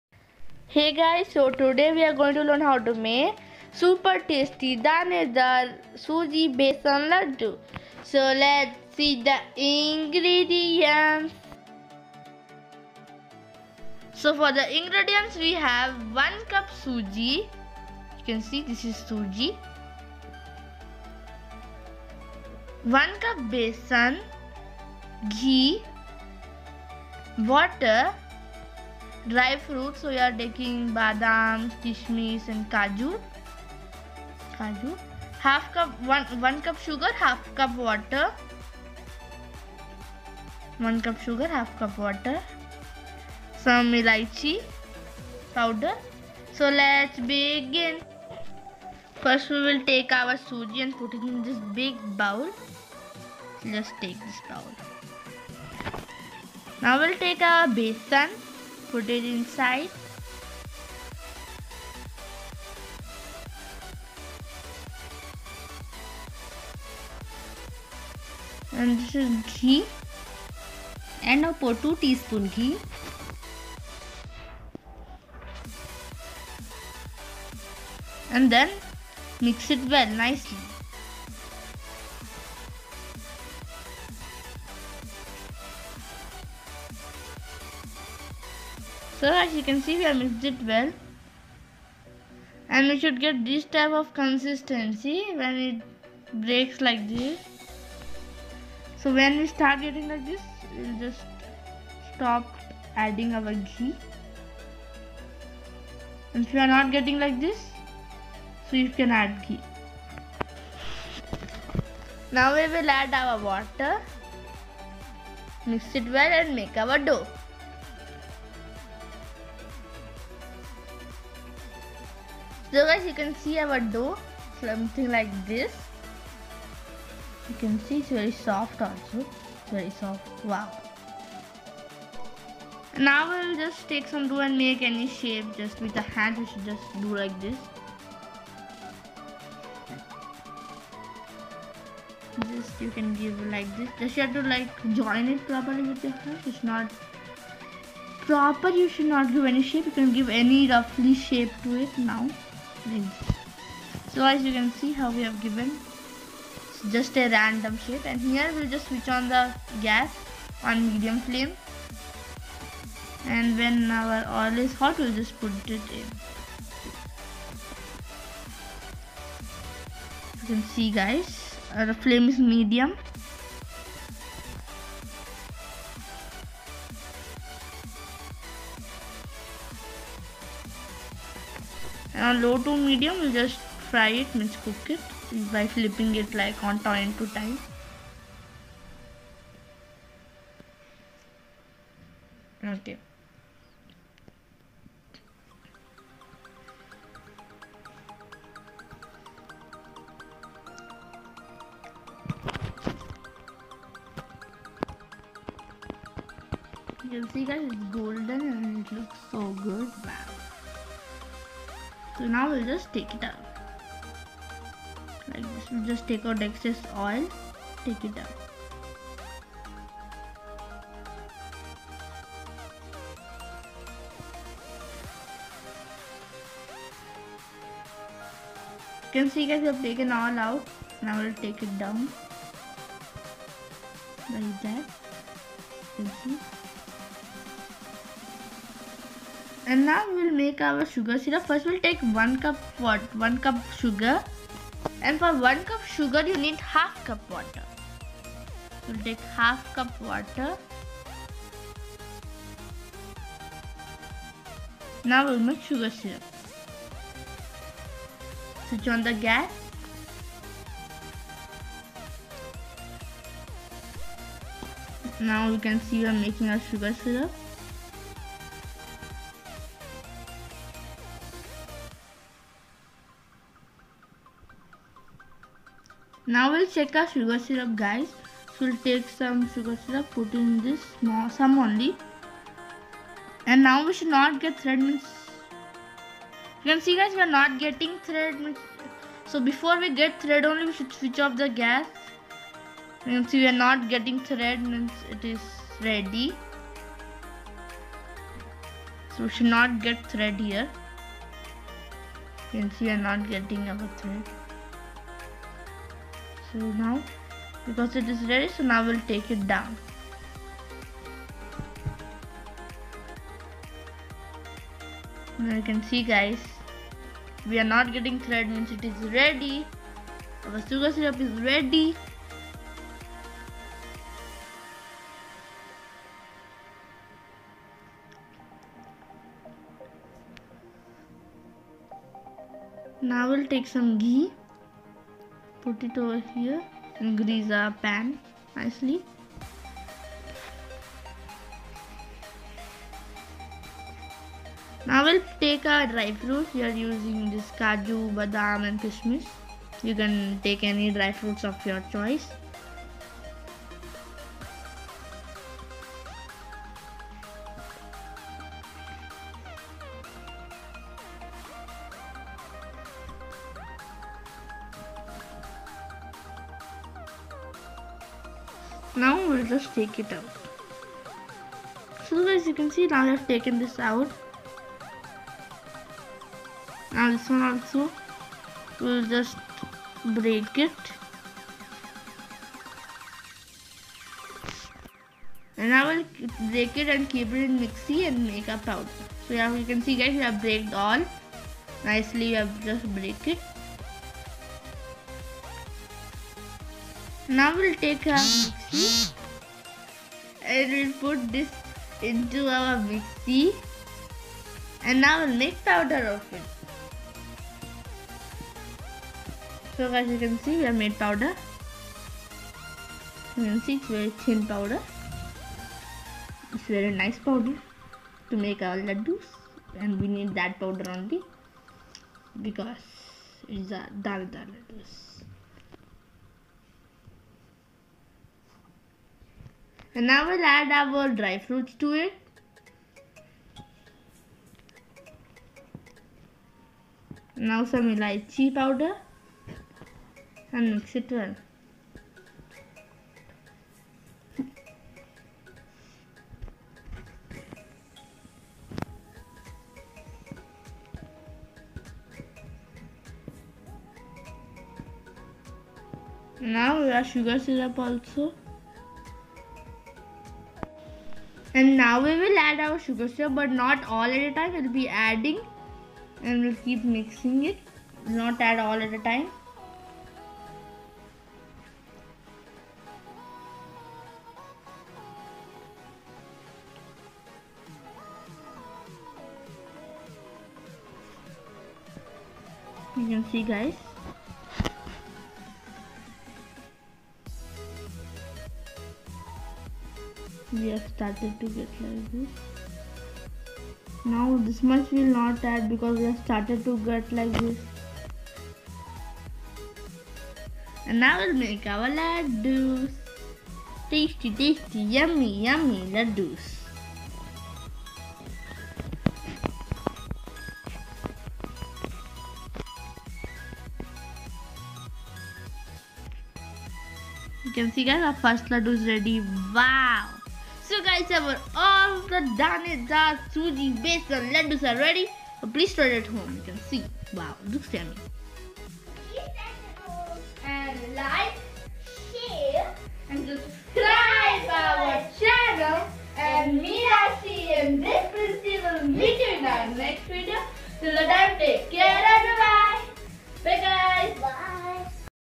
Super tasty danedar suji besan ladoo recipe||Fun and Cook with Rashi and Princy
Today we are going to learn how to make super tasty danedar suji besan ladoo recipe